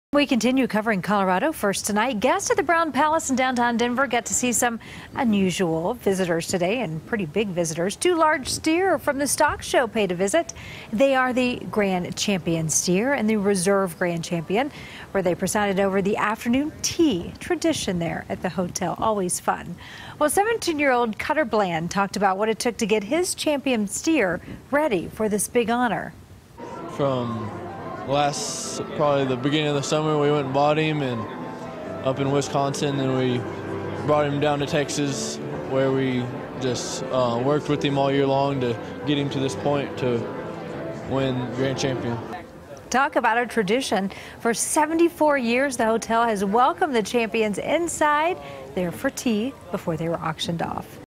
2019年西部牛仔秀冠军牛造访布朗酒店
前天丹佛的布朗酒店（Brown Palace Hotel）延续74年以来的传统，邀请了两位重量级的贵宾到酒店喝下午茶，由于这两位贵宾的驾临，让酒店大厅霎时被挤的水泄不通，围观群众都争相和他们合影。这两位贵宾不是别人，正是今年西部牛仔秀（Western Stock Show）里万中选一的两头冠军牛，总冠军（Grand Champion Steer）净重1339磅，是由一位17岁时饲主饲养，副总冠军（Reserve Grand Champion Steer）净重1369磅，是由一位18岁的寺主饲养，他们浩浩荡荡的到酒店让大家欣赏他们的英姿，尔后，这两头冠军牛分别被以十五万美金和九万美金的天价售出，创下了西部牛仔秀竞标冠军牛的新纪录，竞标所得收益将全部做为支持更多学生就读农业相关科系的助学基金。